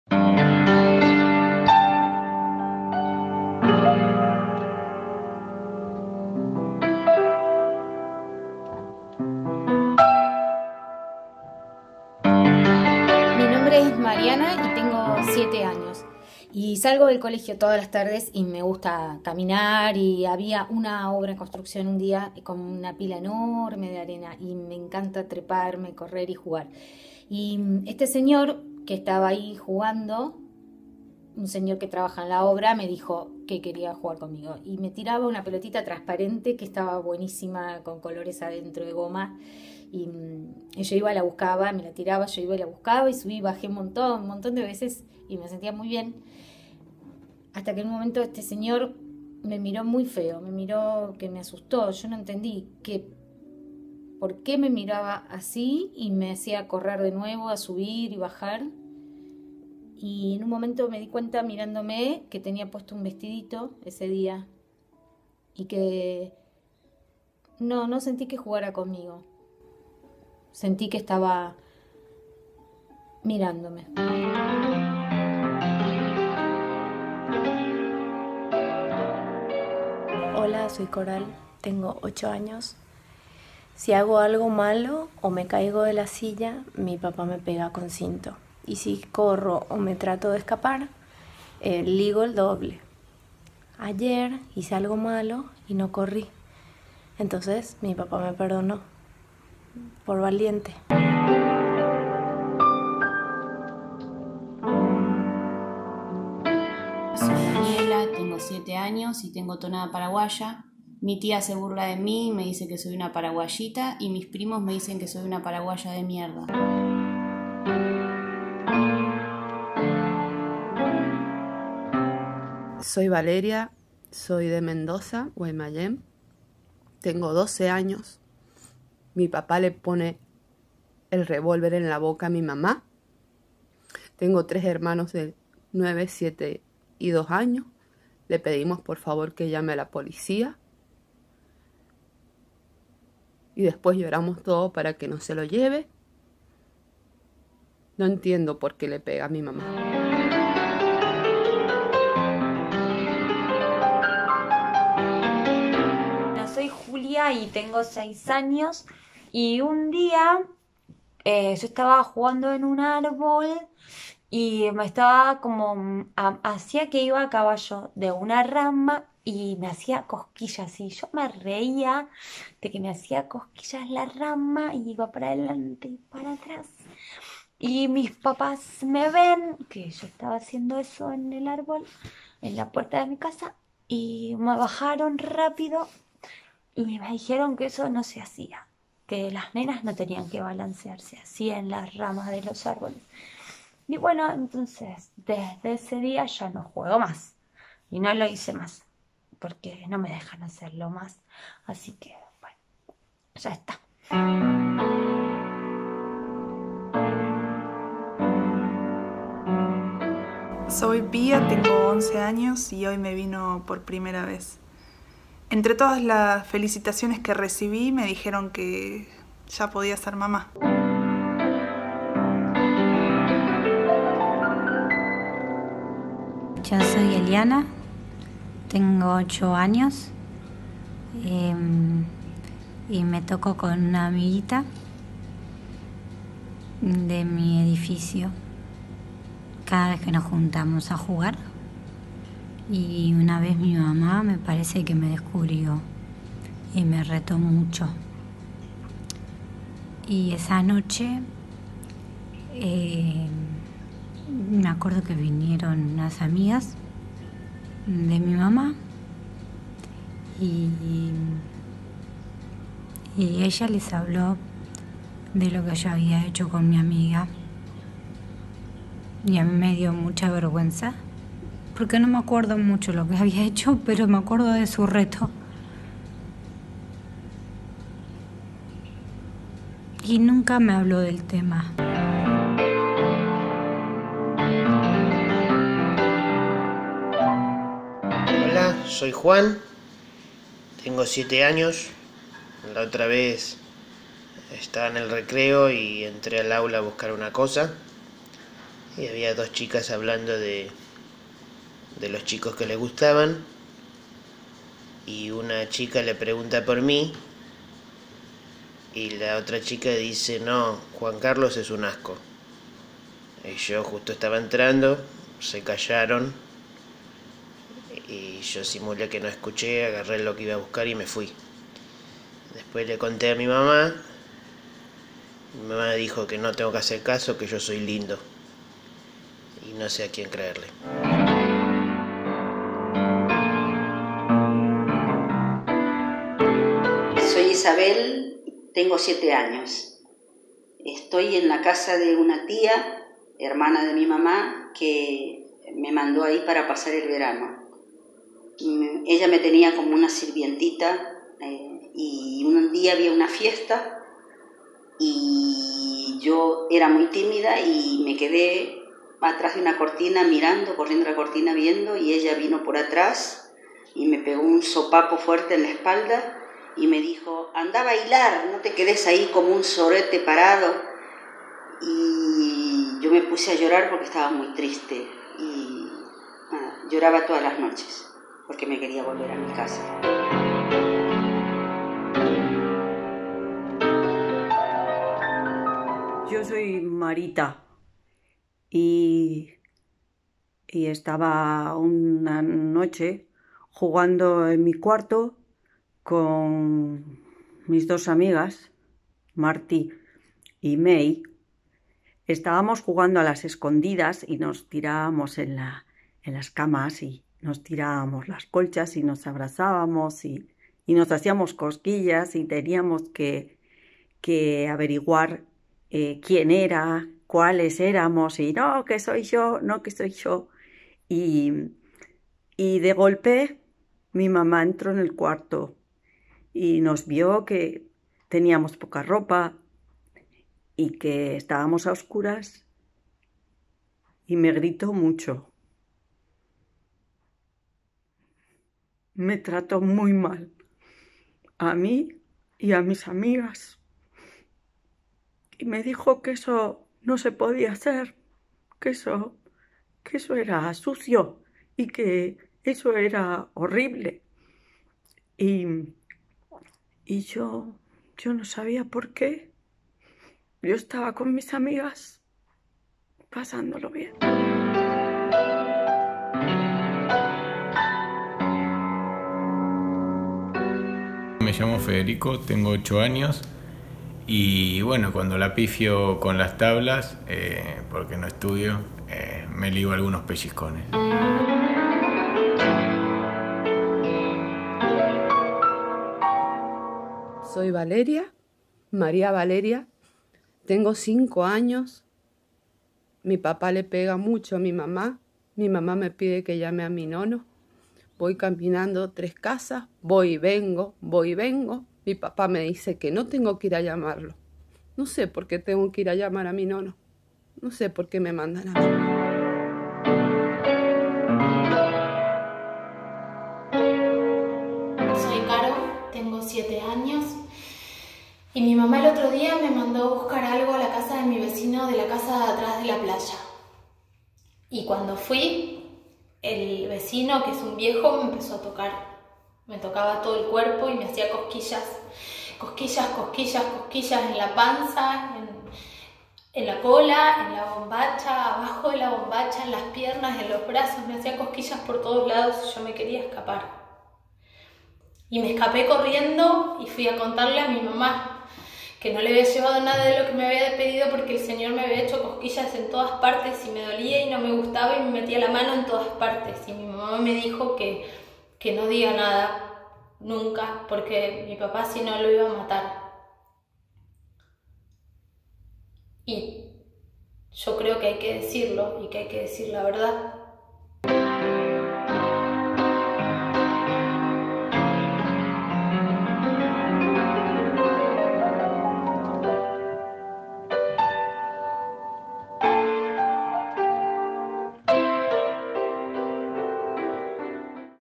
Por unas crianzas libres de violencia
Testimonios en primera persona, sobre situaciones de violencia en las infancias, en una necesidad de develar, contar , denunciar , para ir cambiando esos mandatos que oprimen, dañan, entristecen,  en esos momentos que deberían ser caminos de libertad y aprendizajes. Acción cultural por Che Adelita.